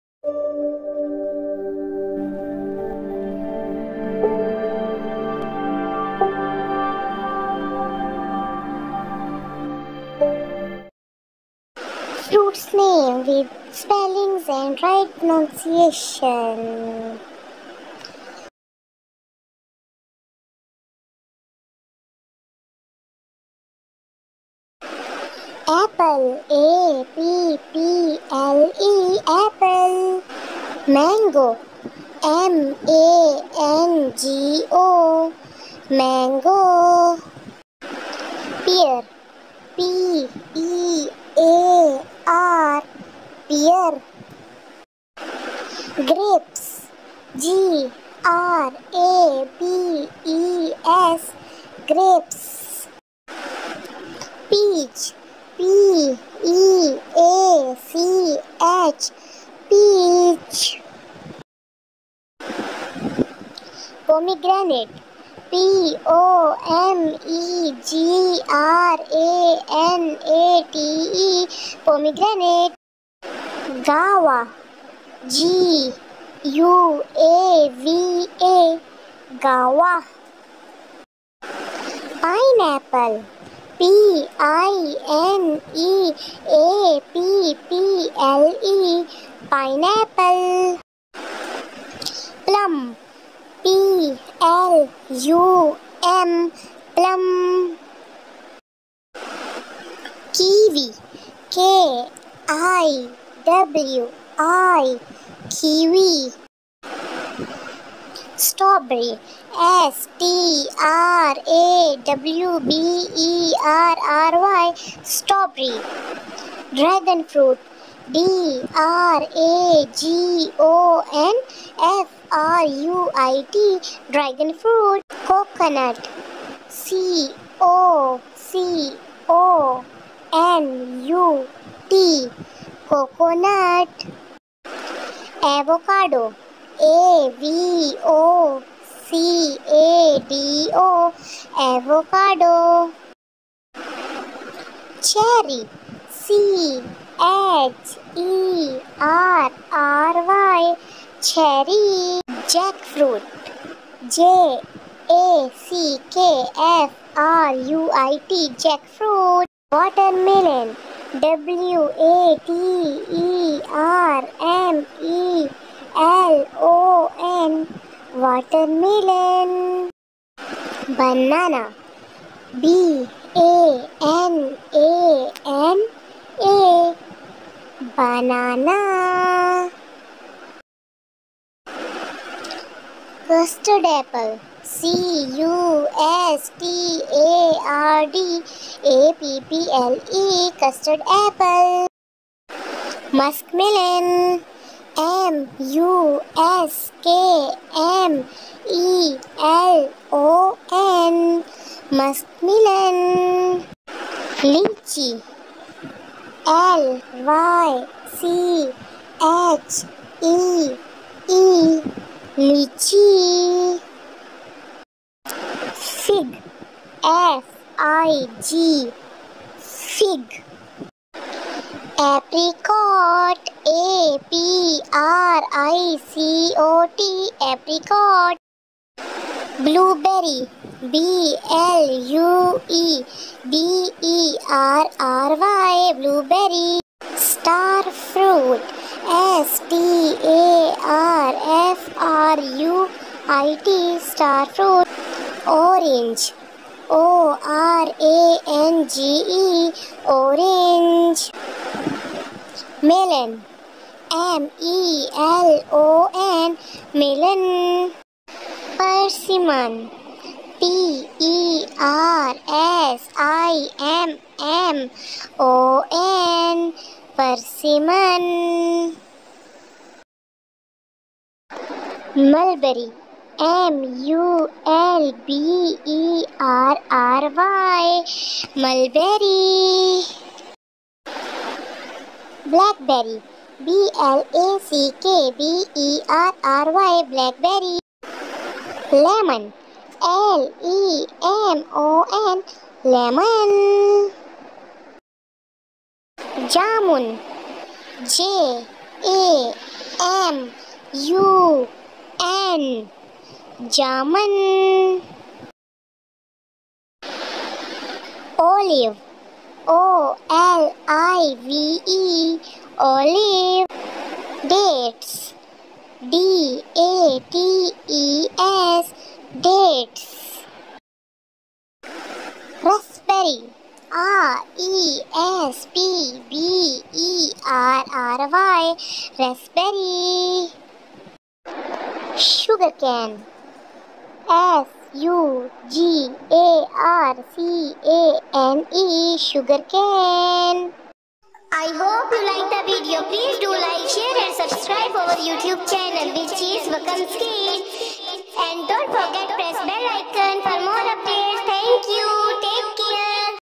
Dancing Fruit's name with Spelling and Right Pronunciation in English 35 fruit for kindergarten Kid
🥭🍍 🍎 🥑 🍇 🍊 🍌 🍉 🍐 🍓🍑 Name & spilling for kindergarten Kid's